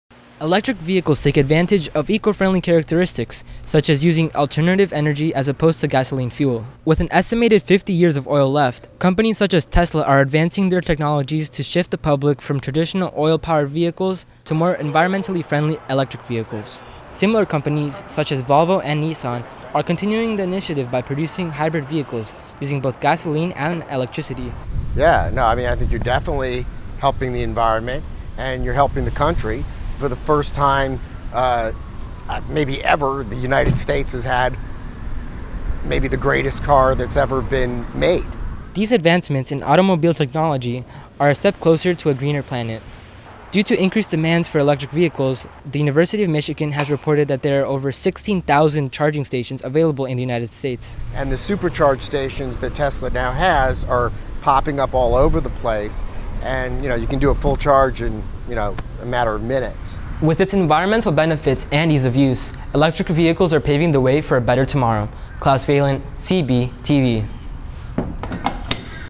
Electric Vehicles
Fossil fuel powered vehicles have been around for more than a century, but how are advancements in Electric Vehicles changing the automobile industry? Find out more in this Electric Vehicle Investigative.

Produced with Alex Tawid and Rhea Thomas